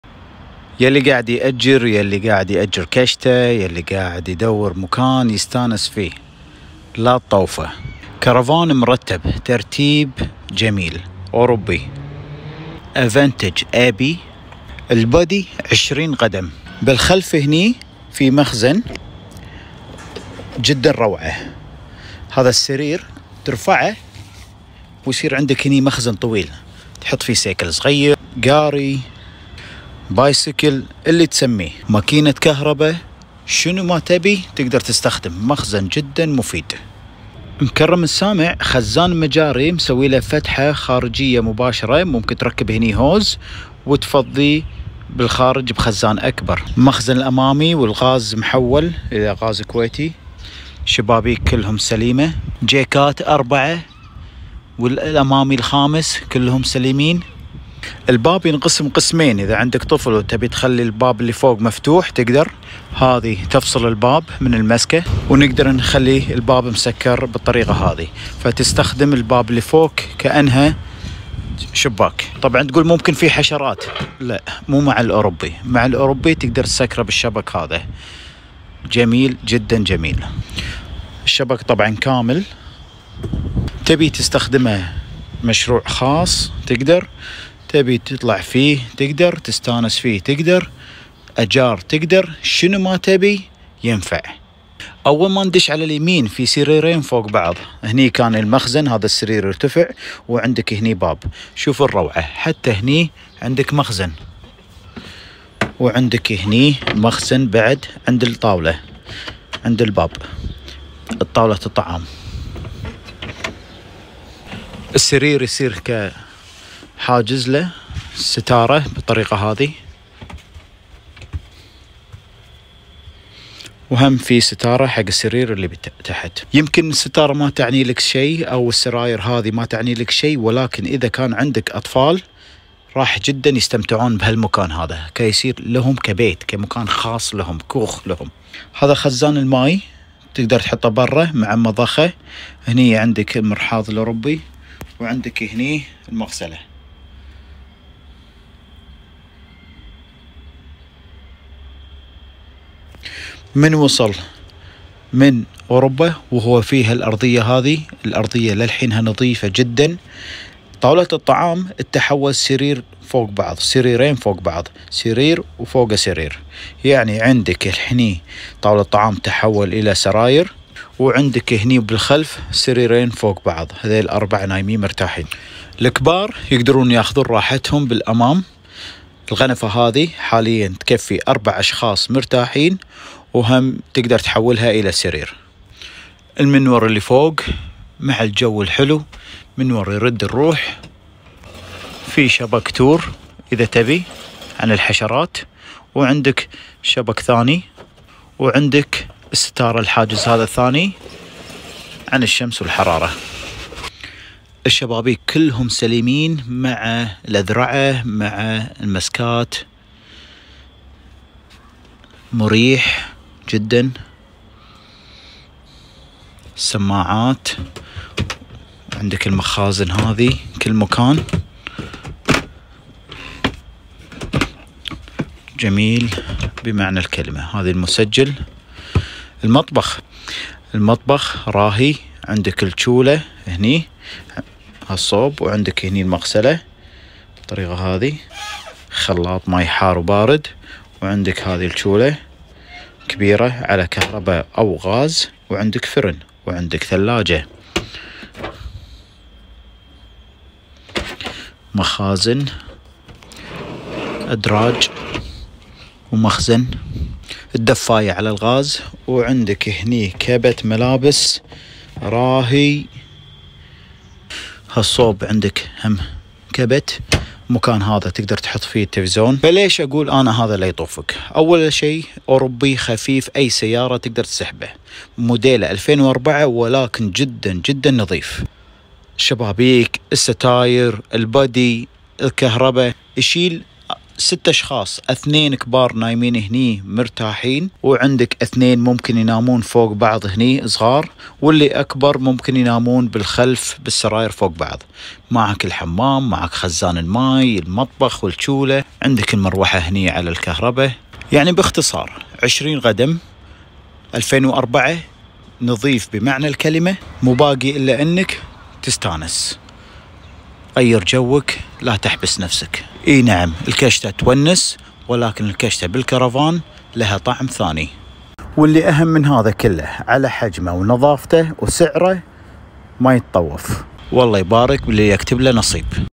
تم البيع اوربي 2004 حجم ٢٠ قدم
تم البيع كرفان اوربي
نوع Abbey Avantage
موديل 2004
حجم ٢٠ قدم
‏يتكون من سريرين فوق بعض في الخلف
‏جلسة في الأمام تكفي أربع اشخاص
‏تتحول إلى سرير لشخصين كبار
‏طاولة الطعام لشخصين
‏تتحول إلى سرير لشخصين فوق بعض
‏بطارية
‏ثلاجة على الغاز والكهرباء
‏سخان ماء على الغاز والكهرباء
‏مخزن خارجي كبير
‏دورة مياه

للاستفسار أرسل وتساب What’sApp